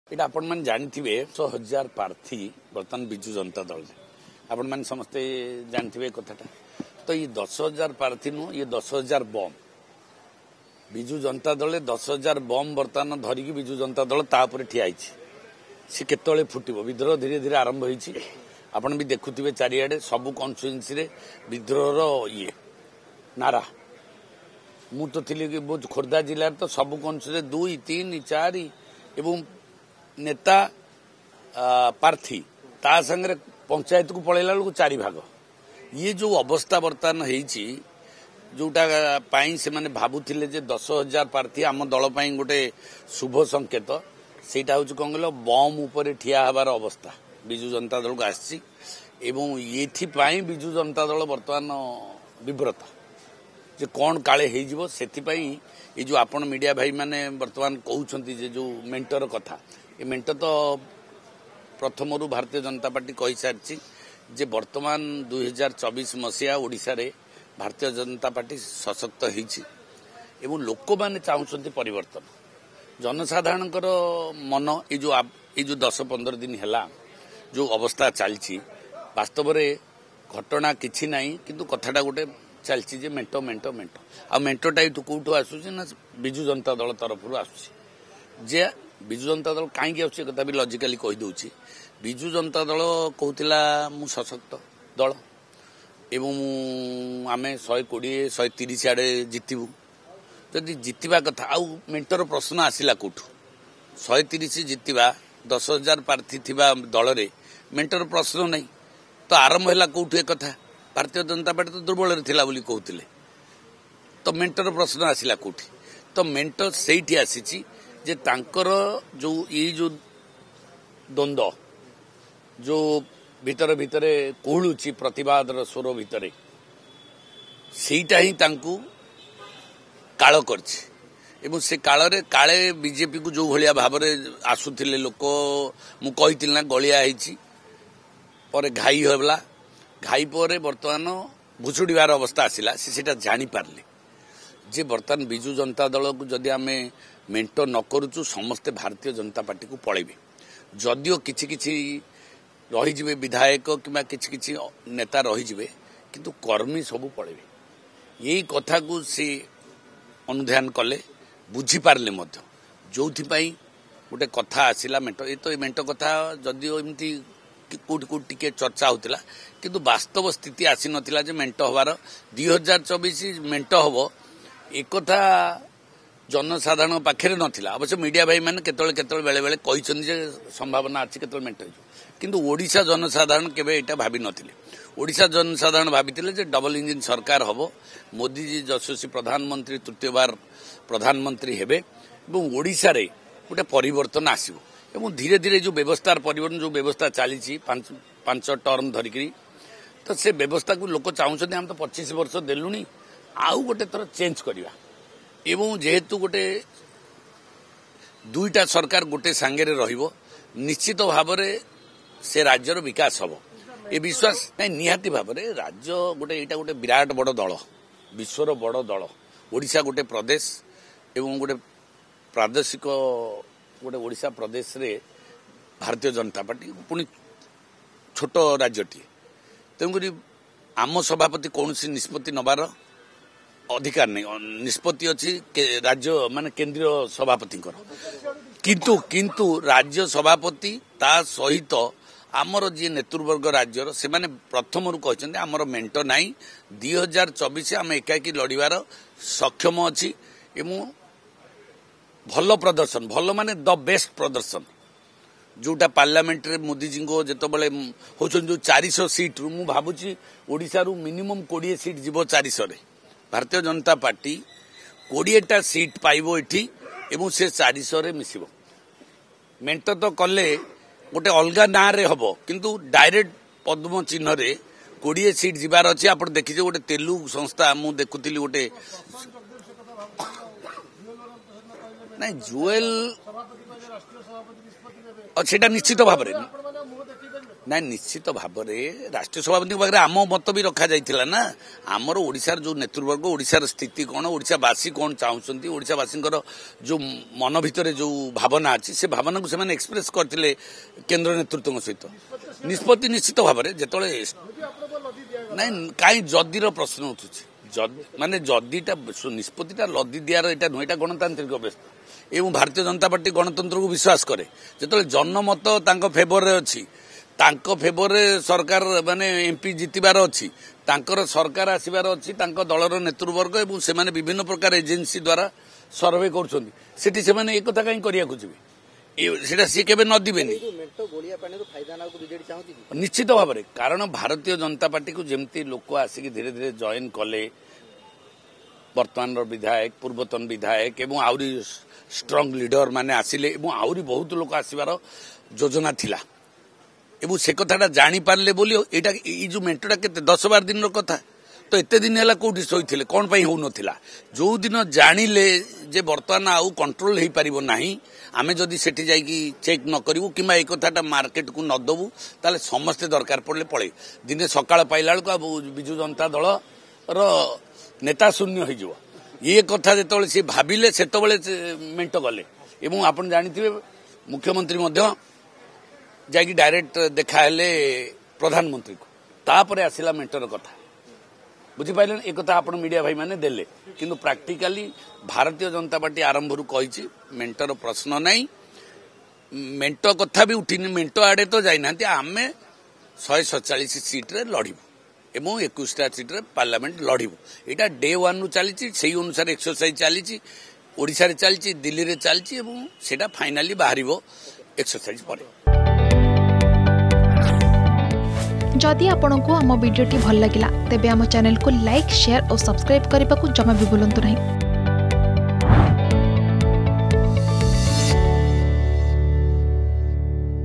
ମେଣ୍ଟ ଦ୍ଵନ୍ଦ ବିଜେଡି ତିଆରି କରିଥିଲା || BJP-BJD Odisha Alliance
ମେଣ୍ଟ ଦ୍ଵନ୍ଦ ବିଜେଡି ତିଆରି କରିଥିଲା : ପ୍ରଶାନ୍ତ ଜଗଦେବ, ବିଜେପି ନେତା ତଥା ଚିଲିକା ବିଧାୟକ